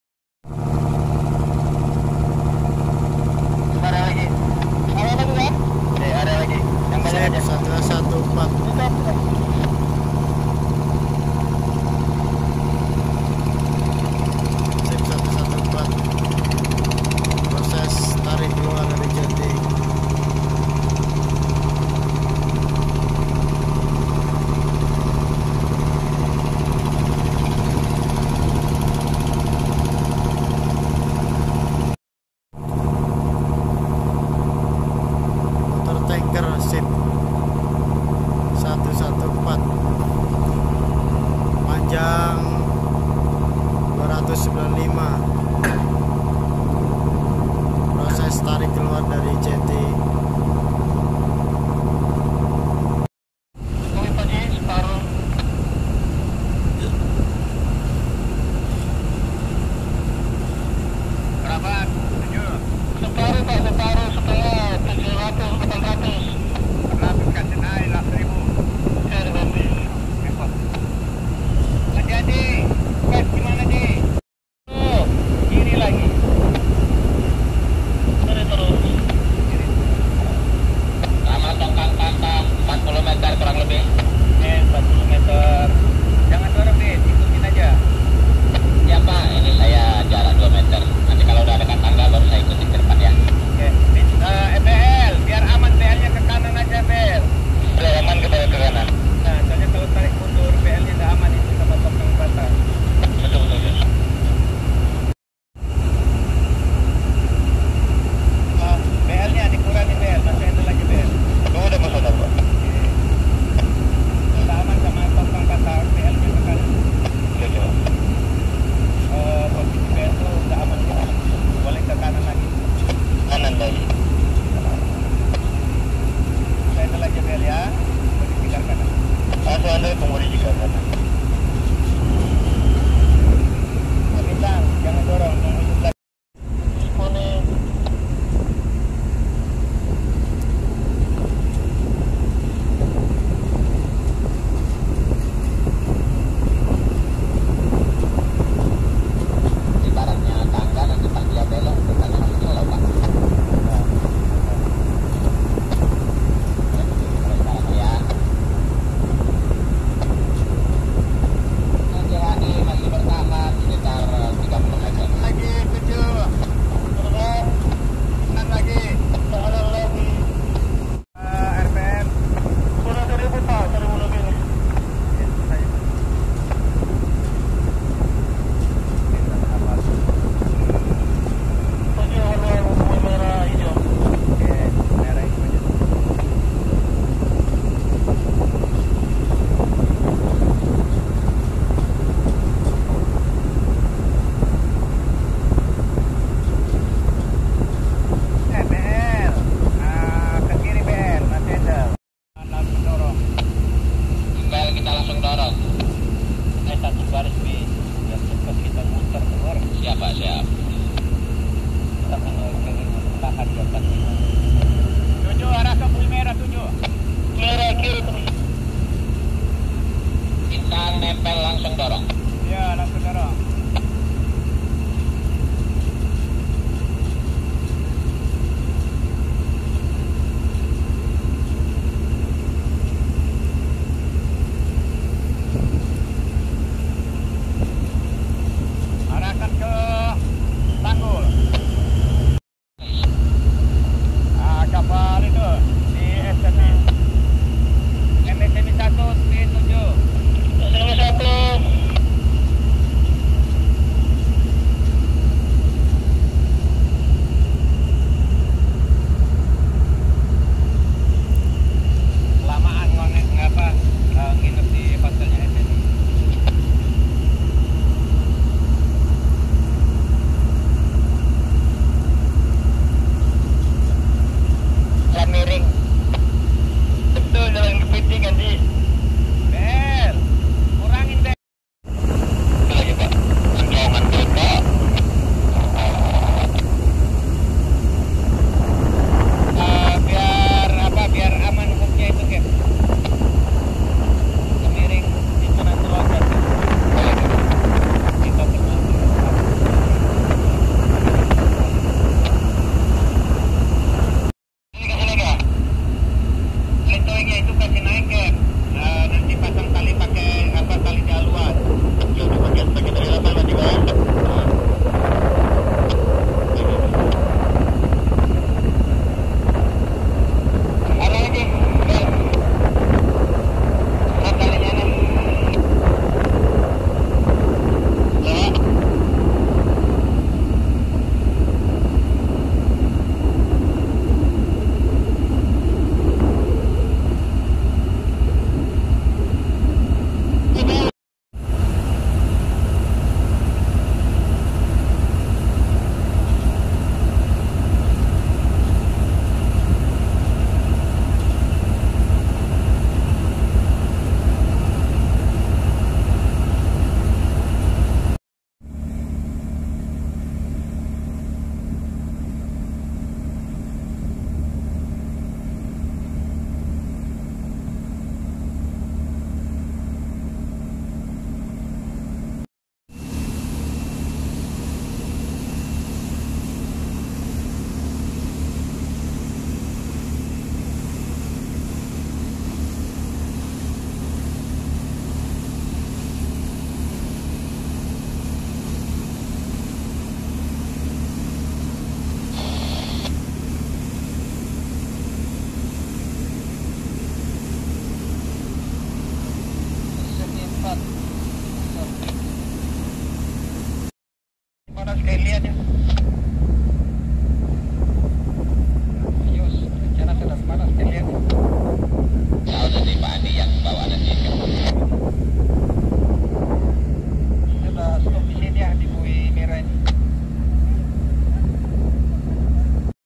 Ship 114 ||kapal tanker penampung minyak di tengah laut
kapal tanker ship 114 cast off dari jety ditarik keluar menuju laut